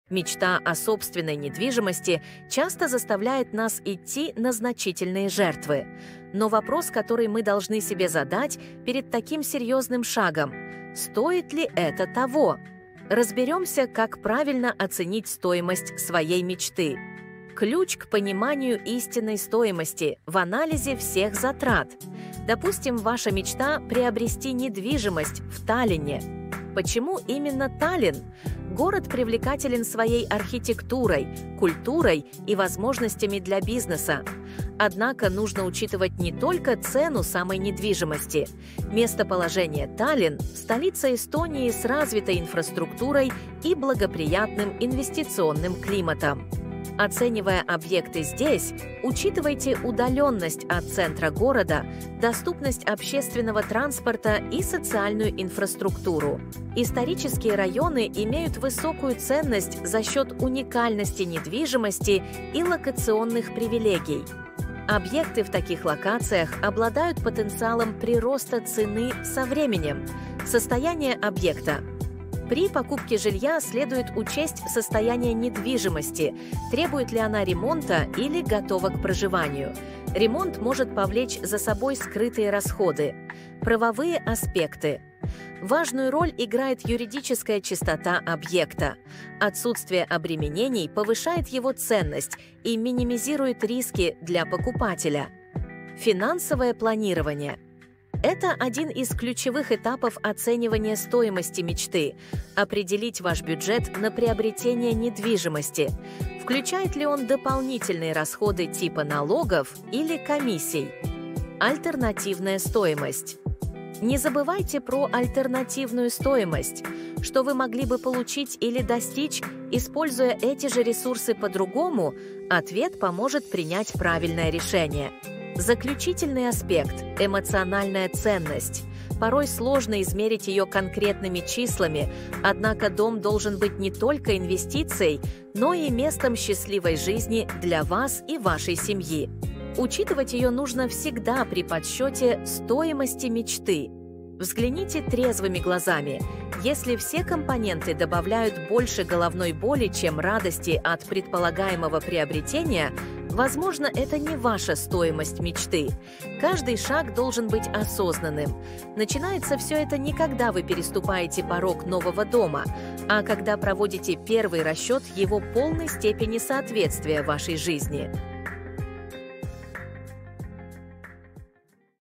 Настоящая стоимость мечты: как правильно оценить то, что вы покупаете
Мечта о собственной недвижимости часто заставляет нас идти на значительные жертвы. Но вопрос, который мы должны себе задать перед таким серьезным шагом: “Стоит ли это того?” Разберемся, как правильно оценить стоимость своей мечты.

Ключ к пониманию истинной стоимости – в анализе всех затрат. Допустим, ваша мечта – приобрести недвижимость в Таллинне. Почему именно Таллинн? Город привлекателен своей архитектурой, культурой и возможностями для бизнеса. Однако нужно учитывать не только цену самой недвижимости.

Местоположение

Таллинн – столица Эстонии с развитой инфраструктурой и благоприятным инвестиционным климатом. Оценивая объекты здесь, учитывайте удаленность от центра города, доступность общественного транспорта и социальную инфраструктуру.

Историческая стоимость

Исторические районы имеют высокую ценность за счет уникальности недвижимости и локационных привилегий. Объекты в таких локациях обладают потенциалом прироста цены со временем.

Состояние объекта

При покупке жилья следует учесть состояние недвижимости: требует ли она ремонта или готова к проживанию. Ремонт может повлечь за собой скрытые расходы.

Правовые аспекты

Важную роль играет юридическая чистота объекта; отсутствие обременений повышает его ценность и минимизирует риски для покупателя.

Финансовое планирование

Это один из ключевых этапов оценивания стоимости мечты – определить ваш бюджет на приобретение недвижимости. Включает ли он дополнительные расходы типа налогов или комиссий?

Альтернативная стоимость

Не забывайте про “альтернативную стоимость” – что вы могли бы получить или достичь, используя эти же ресурсы по-другому? Ответ поможет принять правильное решение.

Заключительный аспект – эмоциональная ценность; порой сложно измерить её конкретными числами, однако дом должен быть не только инвестицией, но и местом счастливой жизни для вас и вашей семьи. Учитывать её нужно всегда при подсчете “стоимости мечты”.

Взгляните трезвыми глазами: если все компоненты добавляют больше головной боли чем радости от предполагаемого приобретения - возможно это не ваша “стоимость мечты”. Каждый шаг должен быть осознанным: начинается все это не когда вы переступаете порог нового дома, а когда проводите первый расчет его полной степени соответствия вашей жизни.